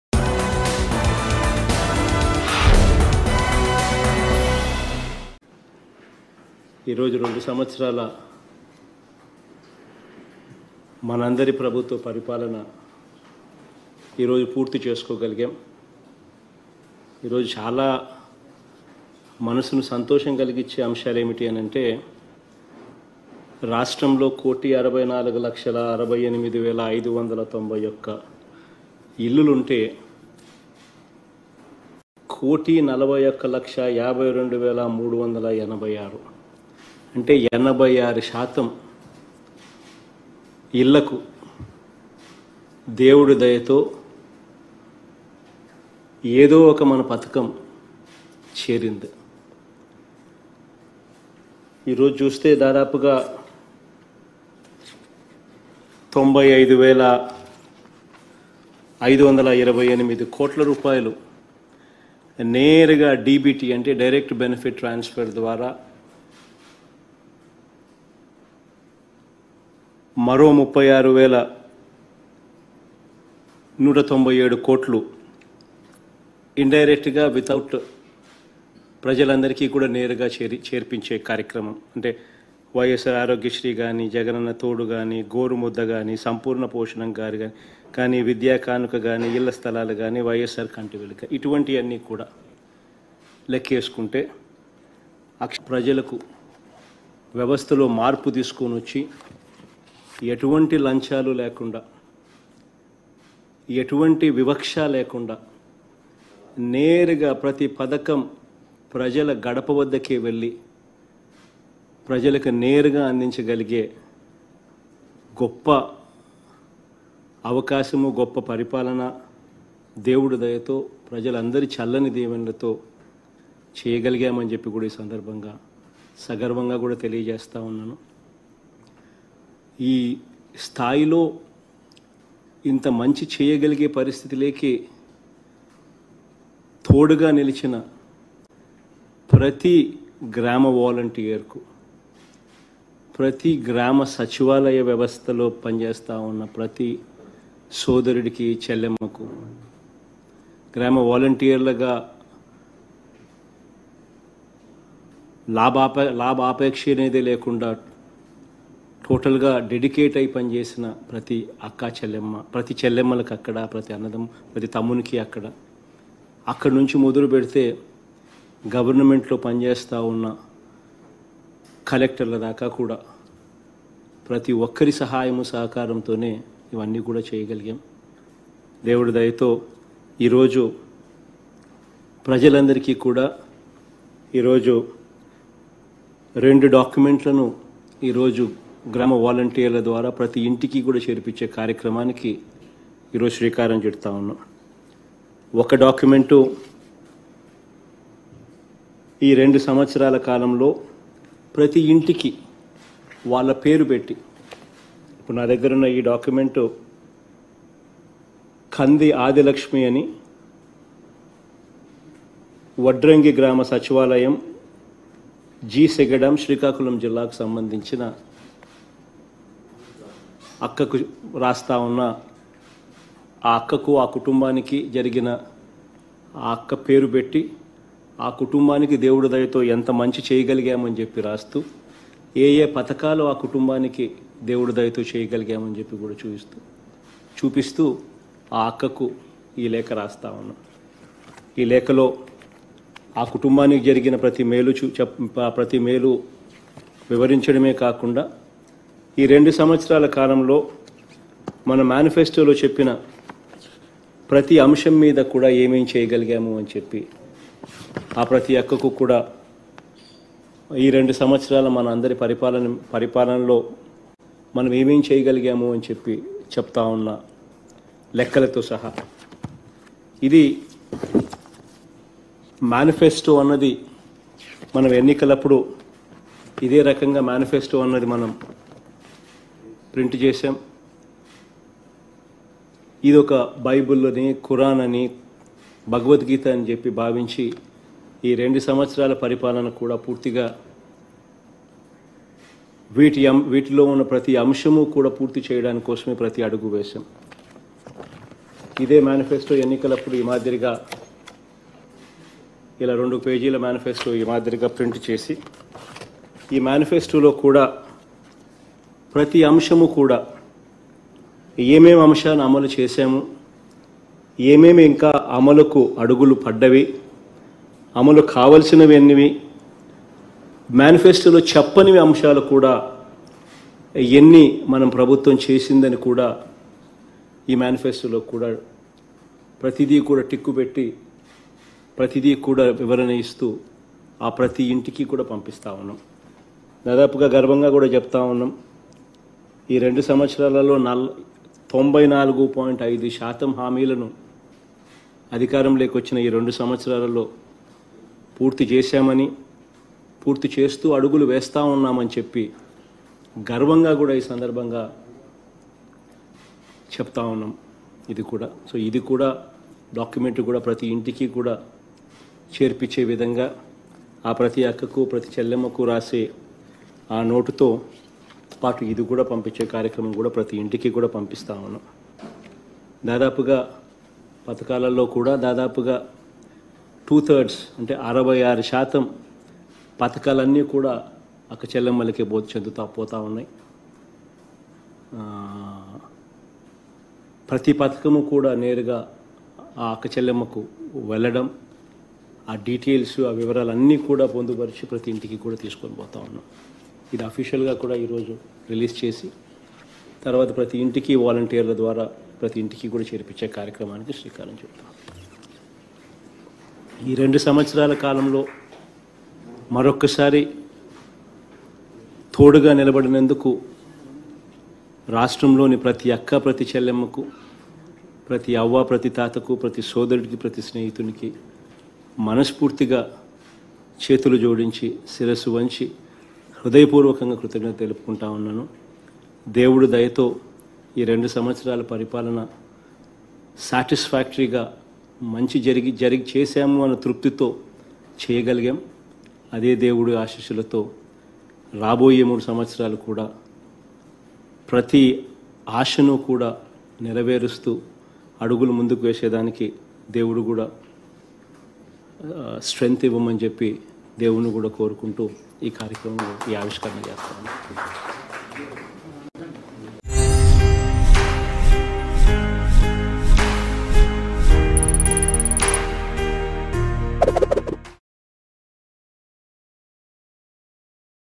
CM YS Jagan Explains About His 2 Years Ruling | AP Govt | YCP | AP Political News | Mango News
CM YS Jagan Explains About His 2 Years Ruling.